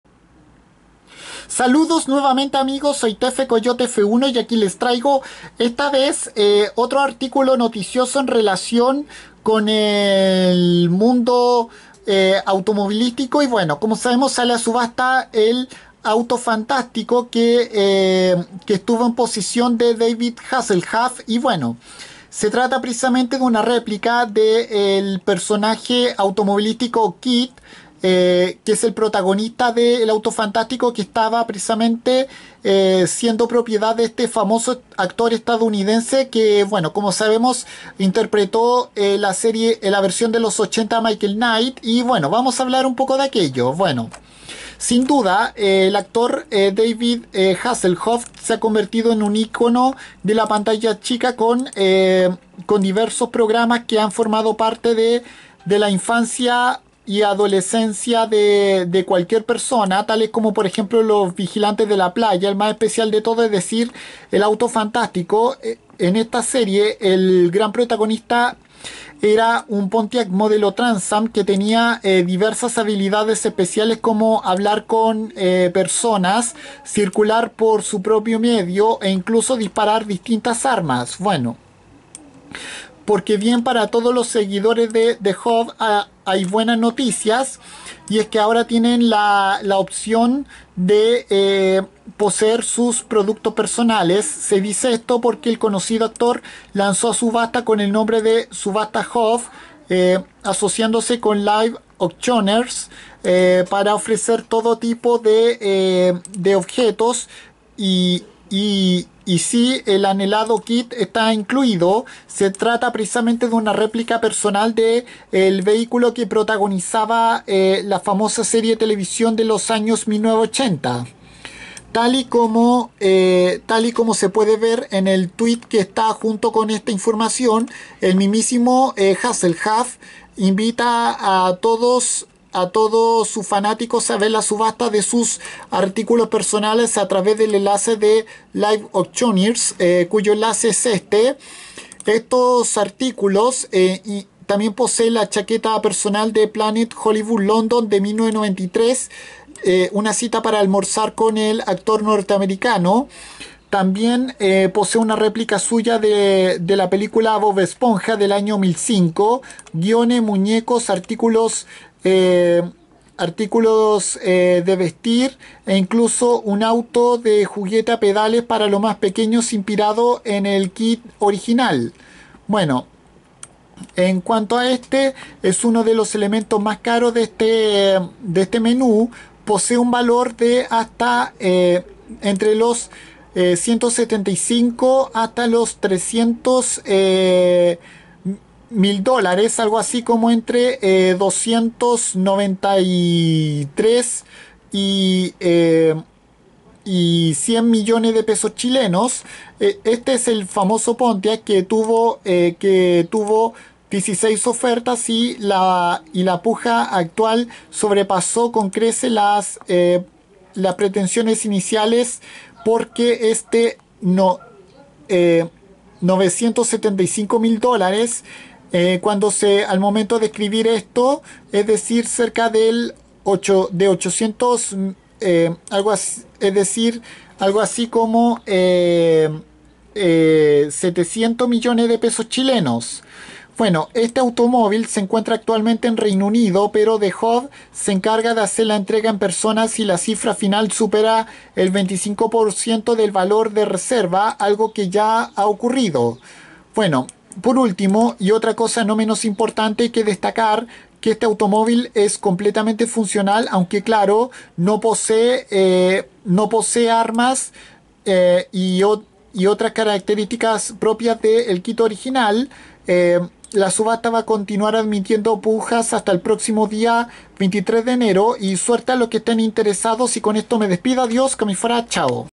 Sale a subasta "El Auto Fantástico" perteneciente a David Hasselhoff
Se trata precisamente de una réplica del automóvil inteligente KITT protagonista de la serie de televisión de los 1980 que hasta ahora era propiedad del mismísimo actor estadounidense